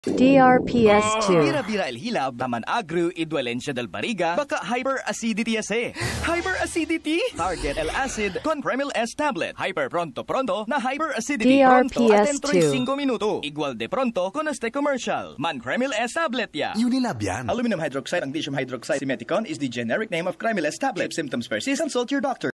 Kremil-S Tablet Radio Ad 2023 22s (Chavacano Version)
Ito po ang Chavacano Version ng 2023 Radio Ad ng Kremil-S, ang antacid brand ng Unilab.

DRPS Two 3345: Kremil-S Tablet Radio Ad 2023 22s (Chavacano Version)